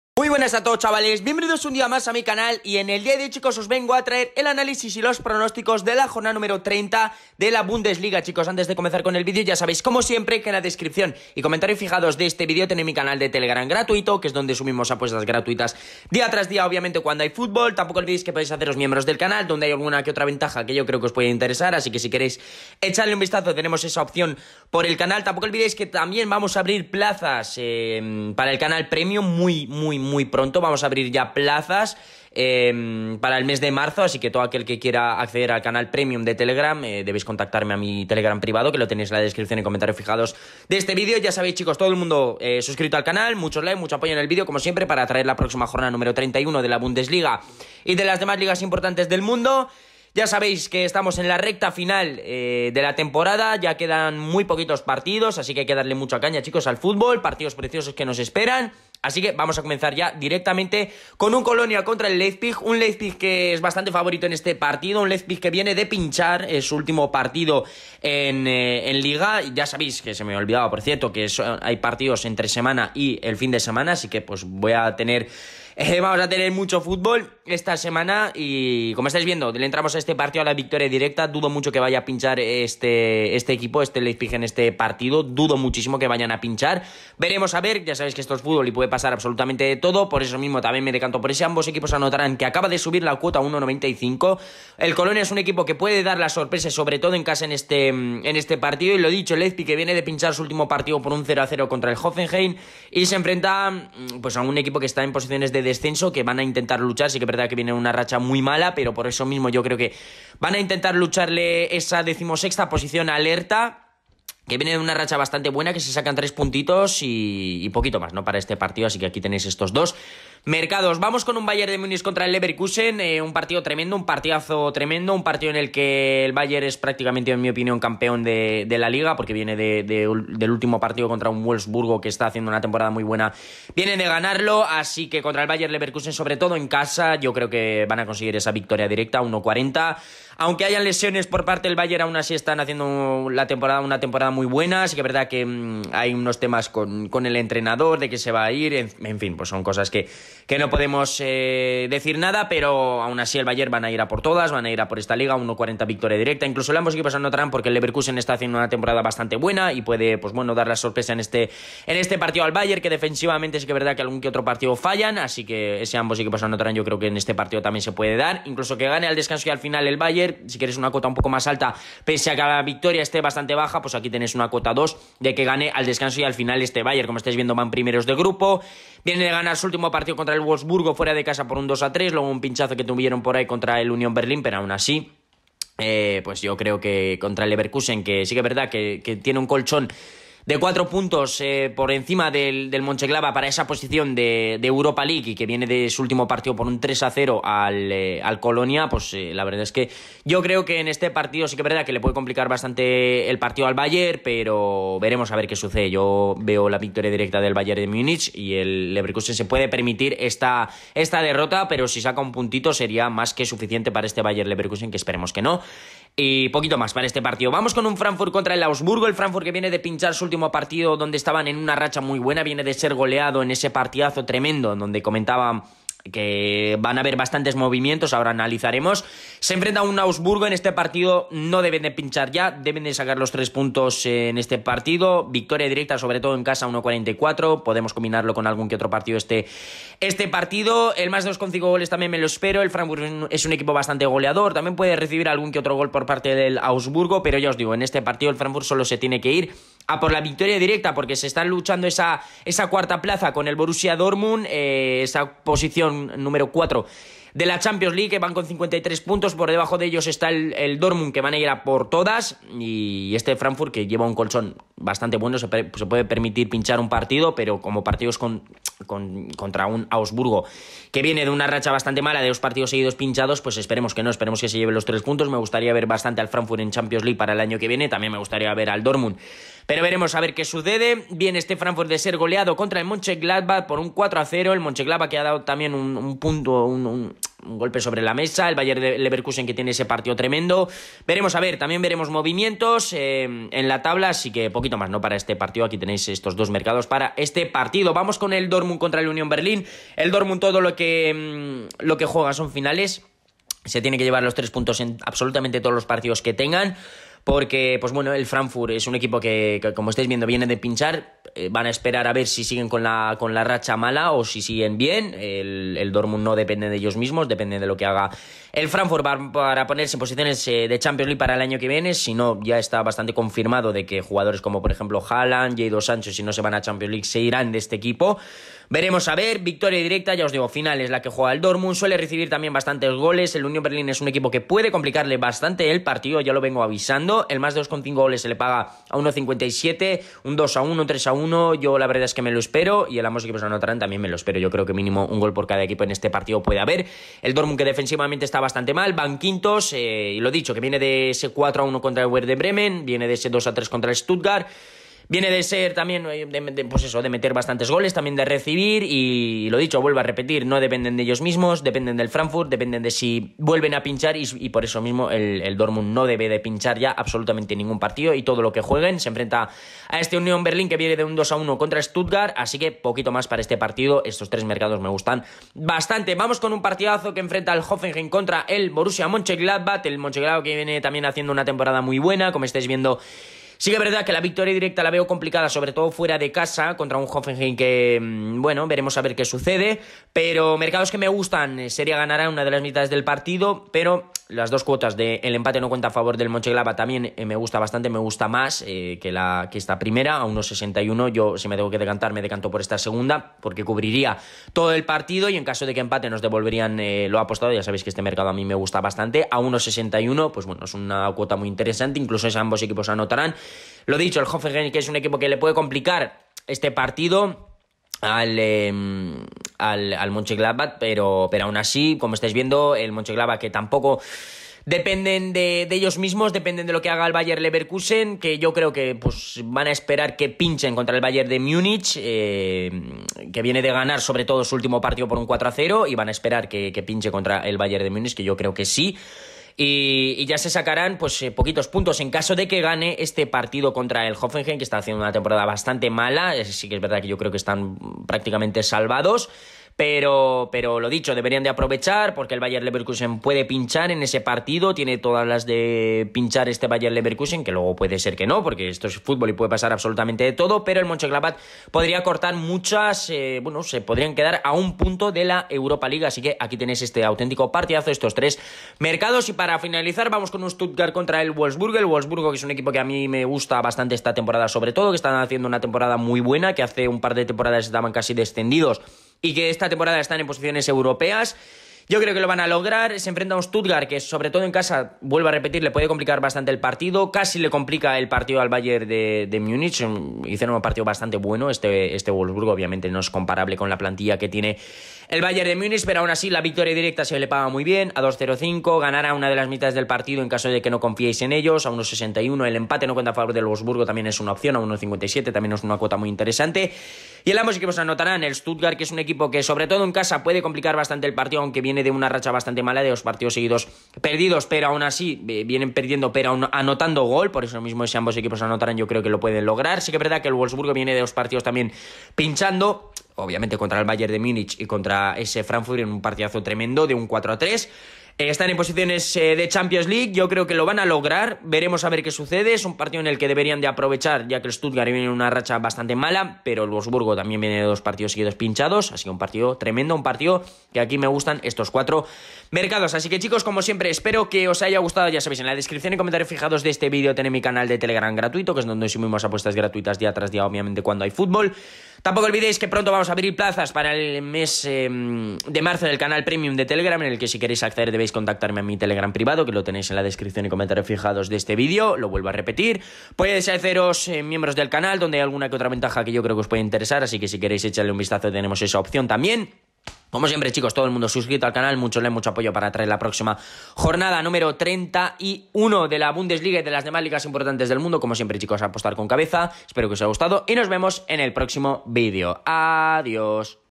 PRONÓSTICOS BUNDESLIGA : JORNADA 31 ⚽ | PARA APUESTAS DEPORTIVAS!!!
ANÁLISIS Y PRONÓSTICOS LIGA ALEMANA BUNDESLIGA JORNADA 31 | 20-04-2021 | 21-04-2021

🔒 PARA ACCEDER AL CANAL PREMIUM ESCRÍBEME POR PRIVADO EN TELEGRAM @CONTACTOTTETESTYLE